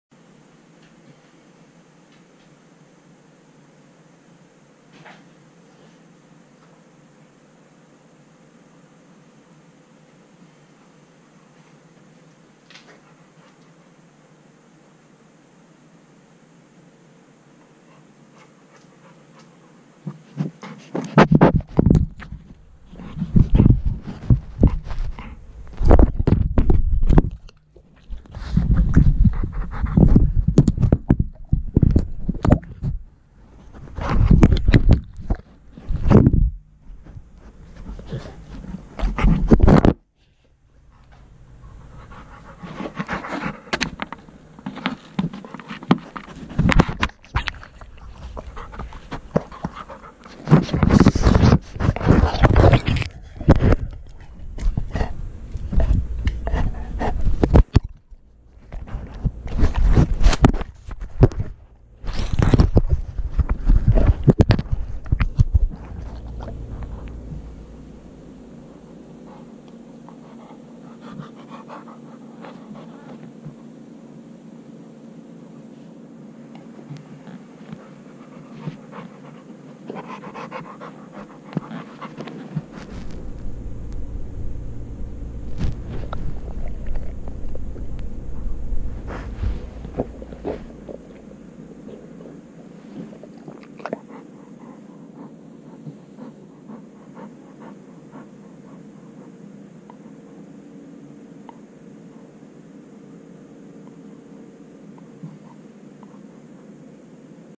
My dog attempted to EAT my GoPro!
Sorry for this NON FPV related video but I was just curious to see what my dog was doing when I left the house. So I put my GoPro session just on the edge of the table by the door where I go out and not even two minutes into the video my dog comes out from the kitchen and sees the GoPro, you can see the rest yourself on video luckily I'll still be able to use the camera for fpv flights and not having to take it into Best Buy to replace it